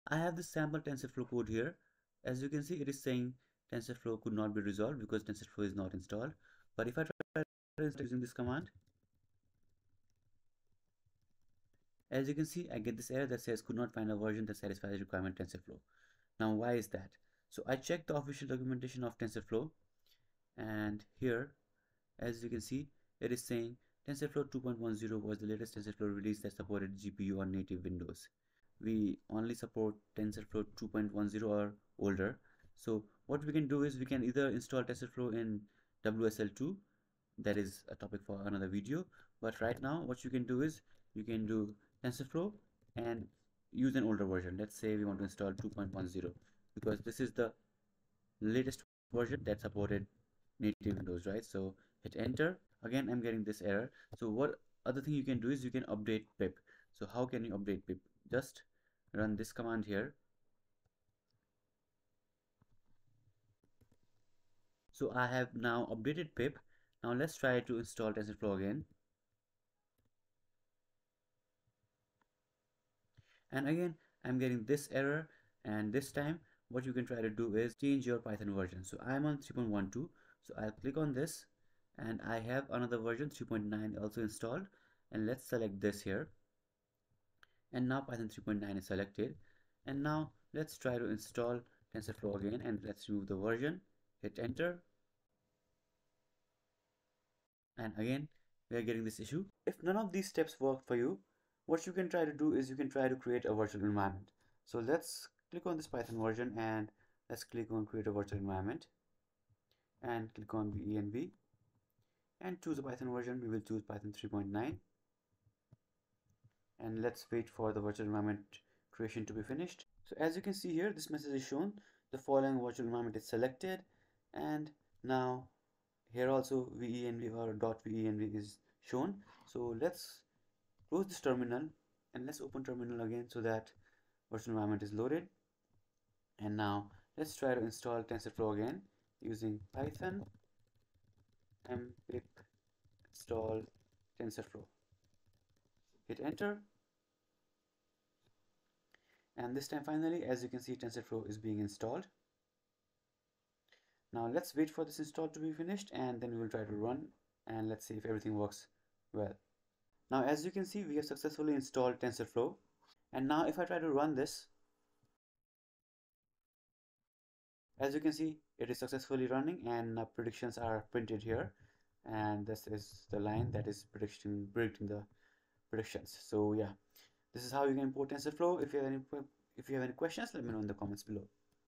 FIXED - Could Not Find a Version That Satisfies the Requirement Tensorflow (2024)
In this video, I'll show you how to fix the error that says: "Cound Not Find a Version That Satisfies the Requirement Tensorflow". No Matching Distribution Found for Tensorflow.
This error occurs when PIP cannot find Tensorflow to download.

By the end of this video, you'll have fixed this issue of PIP not finding tensorflow, and being unable to install tensorflow.